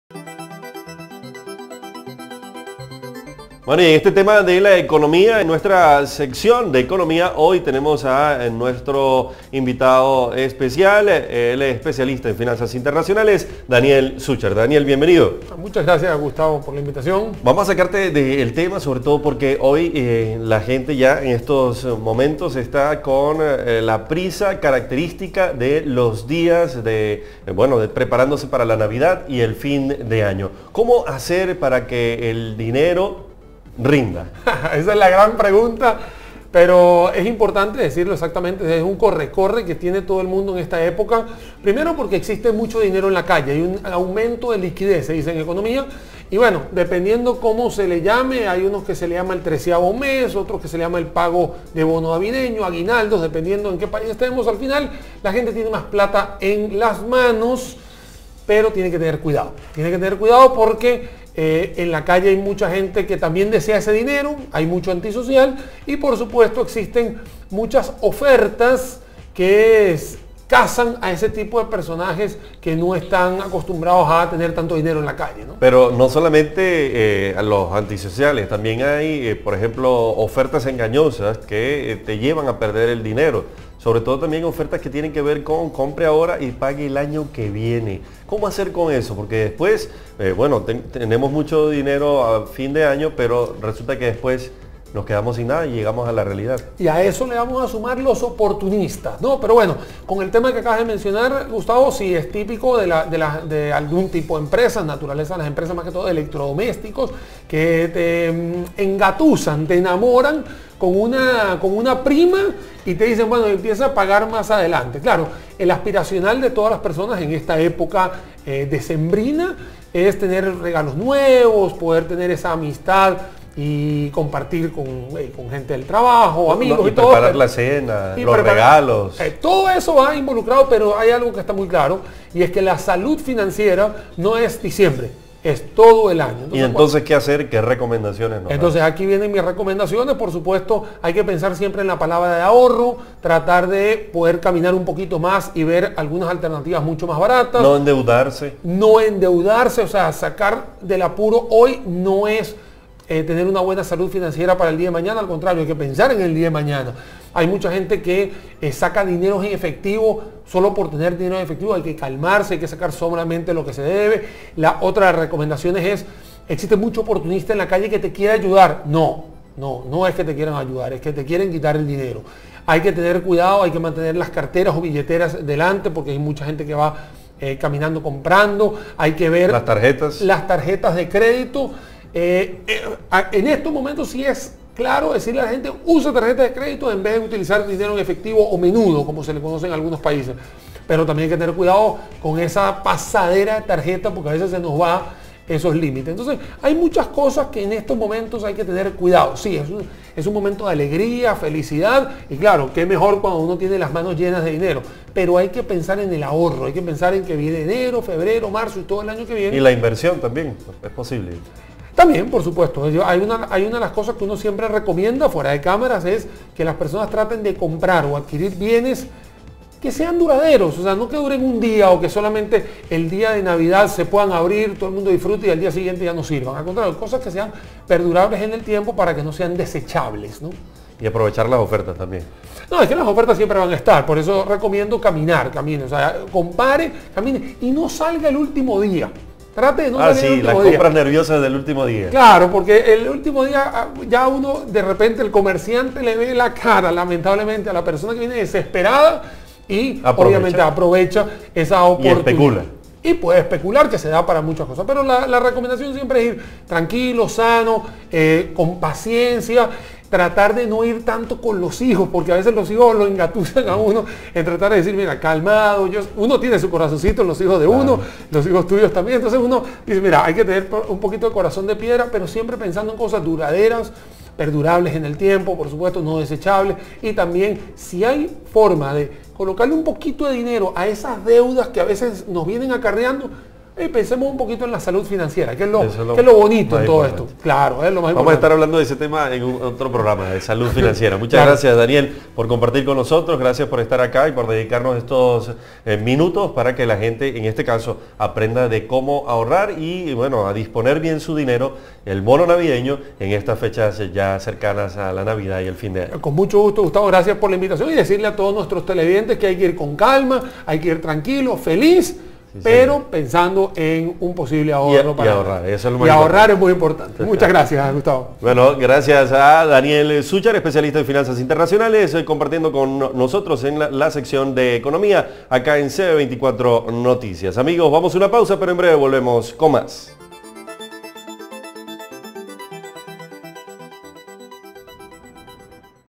Conozca cómo utilizar el bono navideño de una forma eficiente
Diciembre es sinónimo de fiestas y regalos, pero para festejar y poder comprar regalos y no quedarse sin dinero antes de que acabe Navidad es necesario saber administrar los recursos con los que se cuenta en esta época del año.

El especialista en Finanzas Internacionales, Daniel Suchar recomienda en estos tiempos de aumento de liquidez o exceso de dinero en la calle que hay que tener cuidado de los delincuentes y las ofertas de engañosas.

Suchar recomienda caminar, ver y analizar opciones antes de comprar, tratar de generar ahorro y usar con sabiduría las tarjetas de crédito.

A pesar de ser una temporada en la cual la tónica es gastar, el experto en Finanzas Internacionales también recomienda en hacer inversiones que hagan su vida fácil y su bolsillo más solvente para un futuro no muy lejano.

Redacción CB24